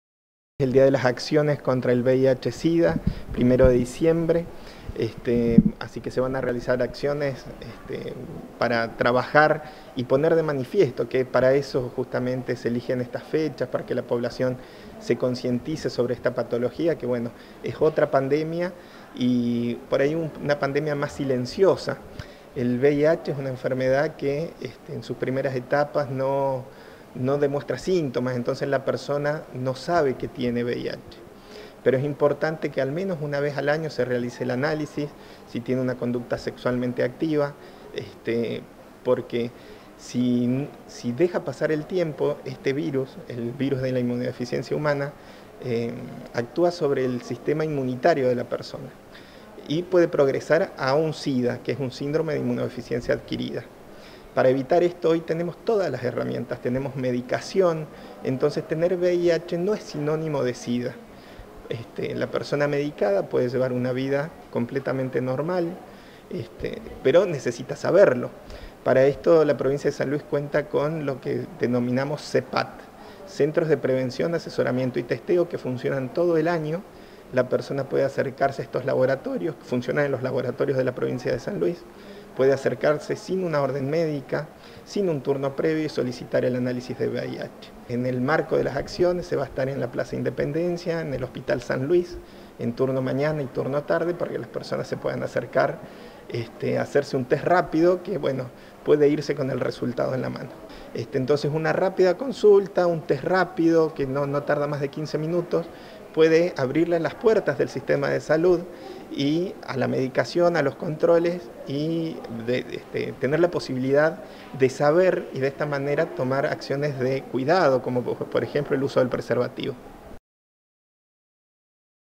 DÍA MUNDIAL DE LUCHA CONTRA EL VIH
El Día Mundial de Lucha contra el VIH que se conmemora este martes 1° de diciembre, en San Luis realizarán testeos voluntarios y gratuitos, respetando la confidencialidad. Quienes deseen realizarse el test, podrán hacerlo en el predio del Hospital San Luis, en la plaza Independencia En La Punta, la sociedad civil “Rock y Vida”.